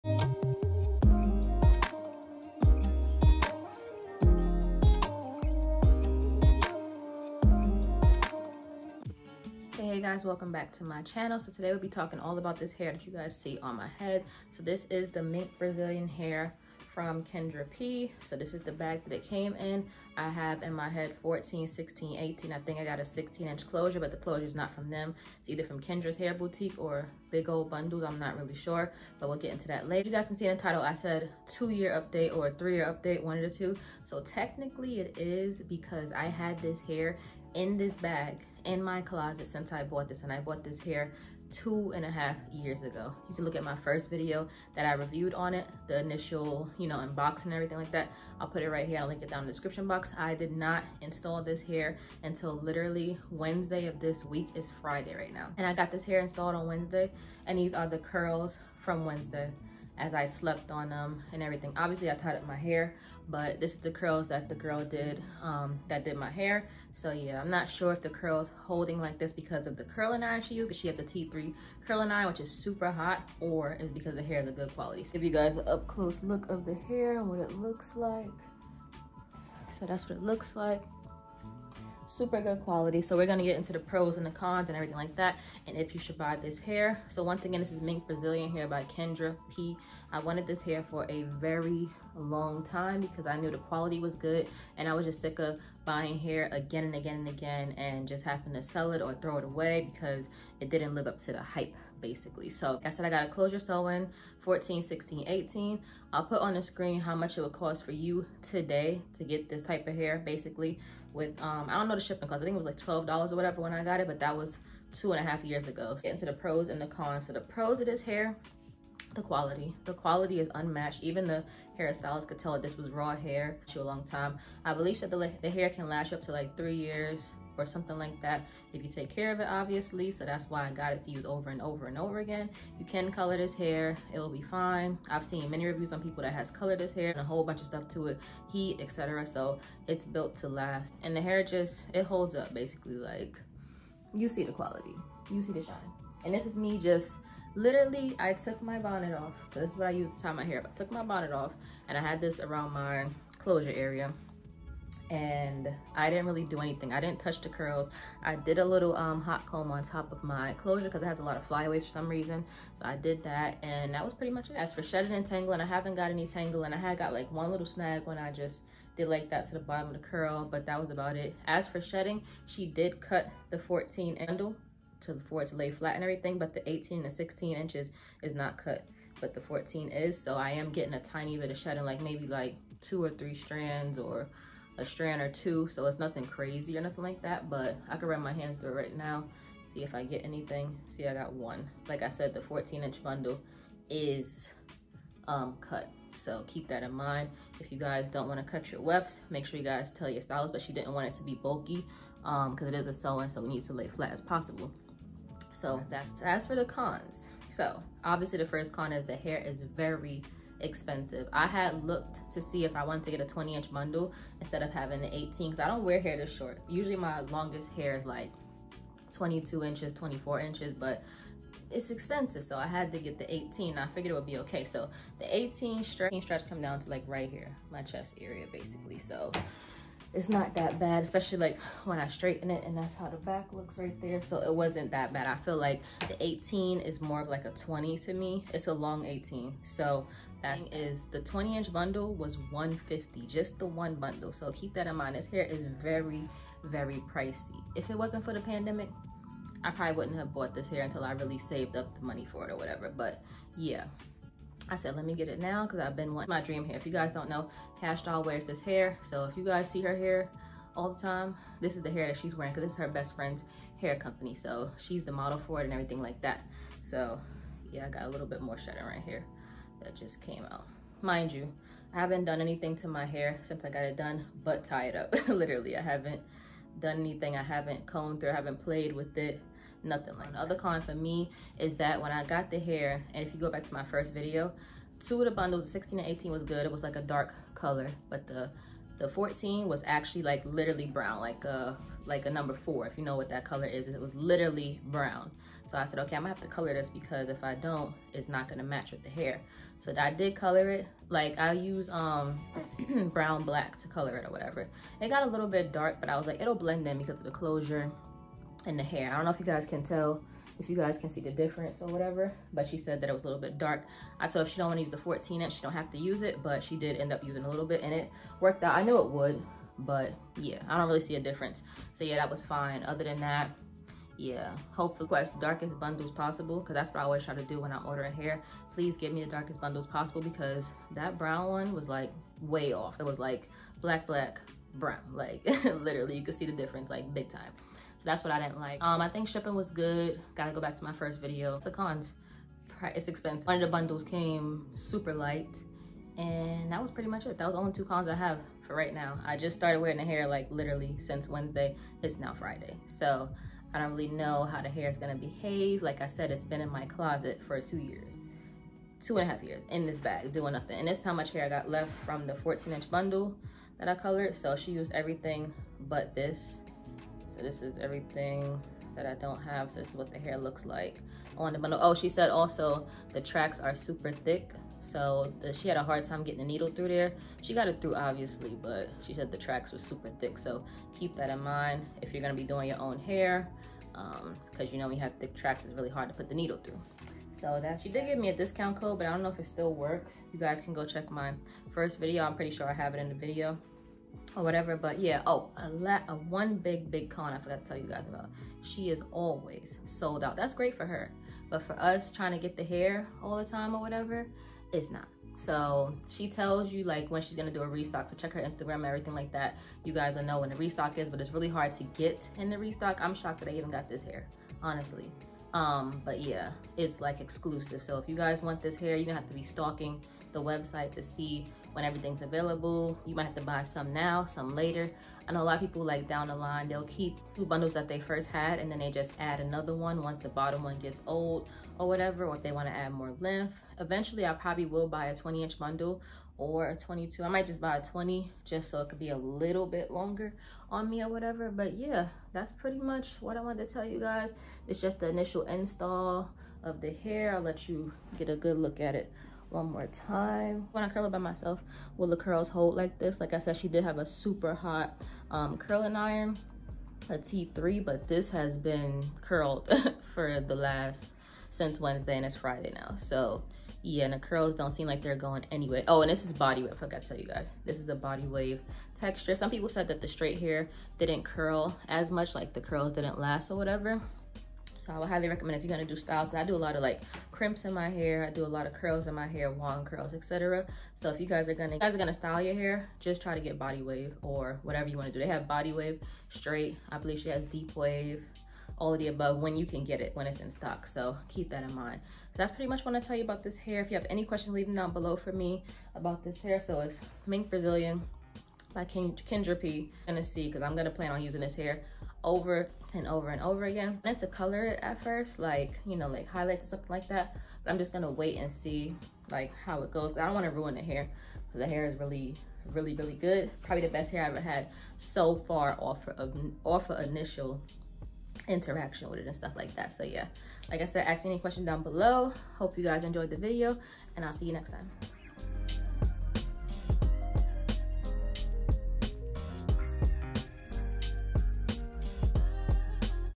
MINK BRAZILIAN HAIR REVIEW | KENDRA P HAIR 2 YEAR INSTALL REVIEW SORTA DO I STILL LIKE IT 🤔🤦‍♀️
TIME STAMPS

Pros
2:00
Cons
3:27
Shedding tangles
4:30